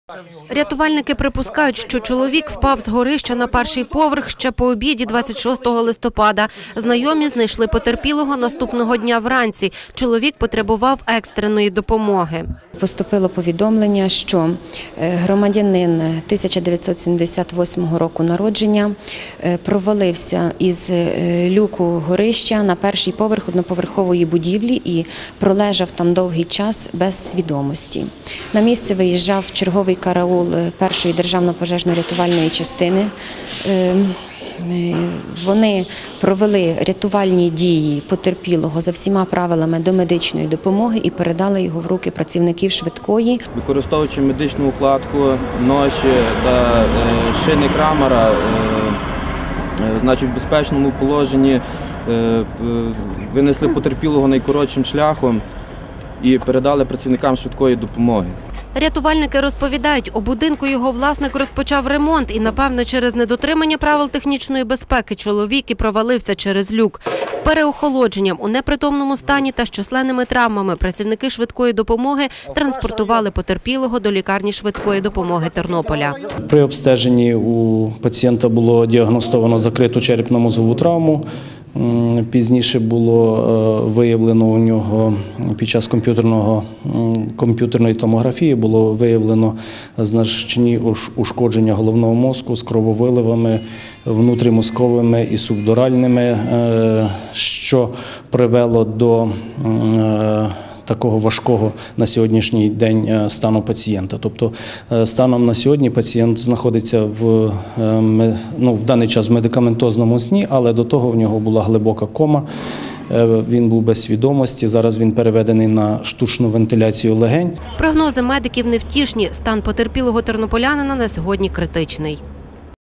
Чоловік впав з горища через люк
34-річний чоловік упав з горища одноповерхового приватного будинку на вулиці Медовій у Тернополі. Чоловік провалився через люк на перший поверх і цілу ніч пролежав там без свідомості. Знайомі знайшли потерпілого вранці 27 листопада і відразу повідомили рятувальників та лікарів. Сьогодні тернополянин знаходиться у лікарні швидкої допомоги, стан його критичний.